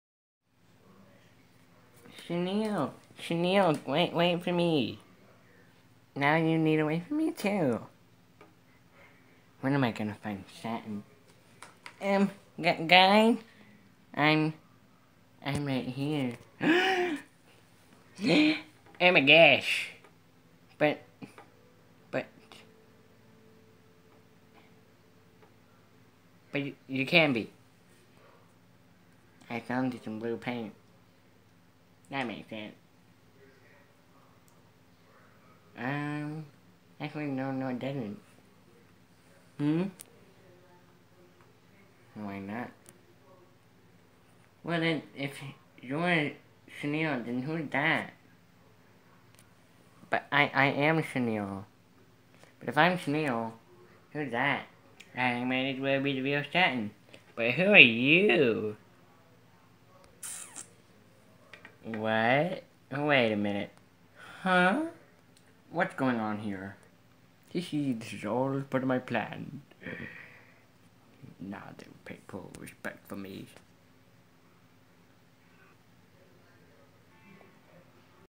TROLLS series 4 fan made commercial
A mini fan made commercial I made

I OWN NONE OF DREAMWORKS
THIS WAS A FAN MADE COMMERCIAL
FOR  THE TROLLS SERIES 4 BLIND BAGS